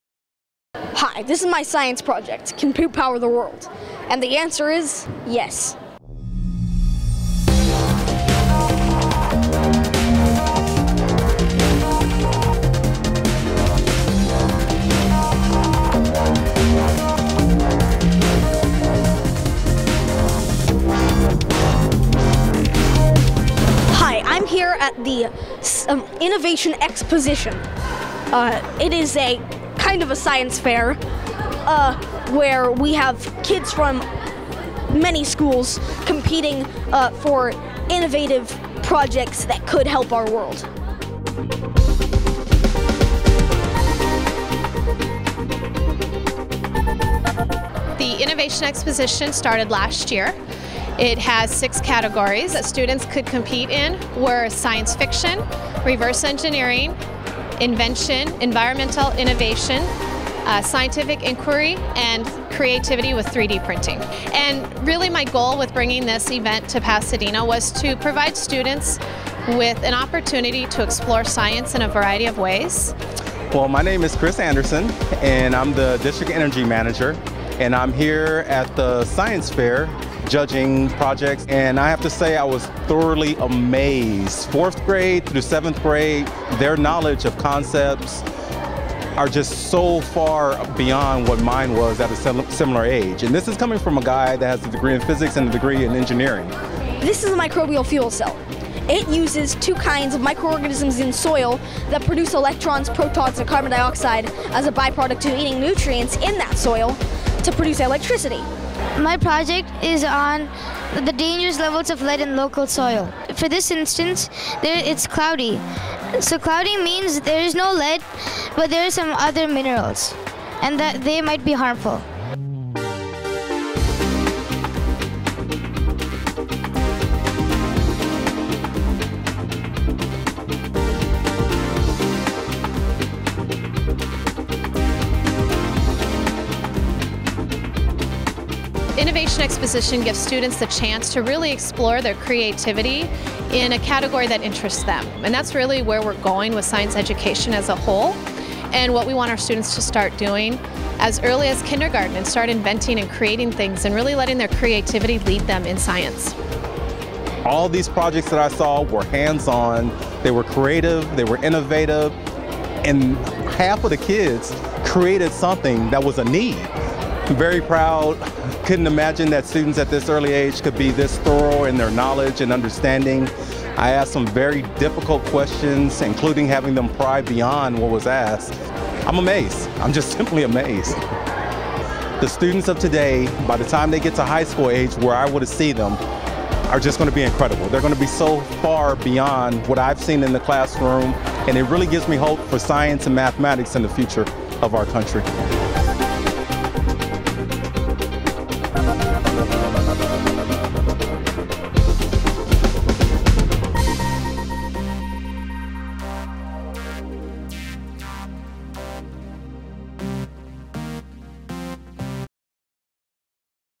PUSD PULSE - Innovation Exposition
The Innovation Exposition is more than just a science fair. It was created to showcase the great STEM work that is happening at our schools in a variety of categories. In order to complete these projects, students needed to combine their content knowledge with creativity and critical thinking.  Then, they spent the morning answering a series of questions asked by STEM experts as they defended their work.  Not an easy task to be sure!  Congratulations to all who participated!